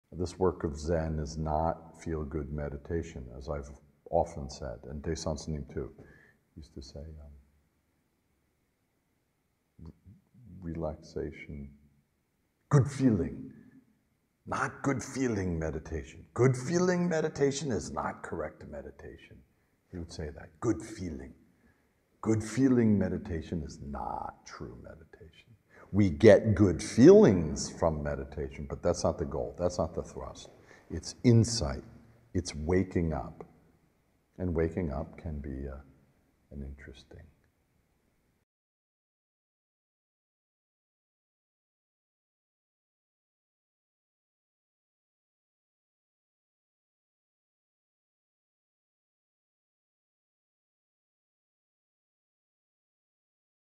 ''Good Feeling'' meditation (insta)
We nearly always experience improved mood and concentration after meditation, especially when we are doing frequent, regular meditation. This good feeling sometimes motivates us to continue the practice: it is a great inducement! But is that resultant "feeling" the reason for which we meditate? Is that the "purpose" for meditation?

In this tiny Instagram mini-bite, Hyon Gak Sunim introduces us to Zen Master Seung Sahn's critique of one of the popular views or uses of meditation, what he derisively called "feel-good meditation." See what his take on that is. But see the longer video for more...

[ Excerpted from a longer talk by Sunim, titled "A Zen Poem by Seung Sahn Sunim"

Support a Monk! Support the Dharma!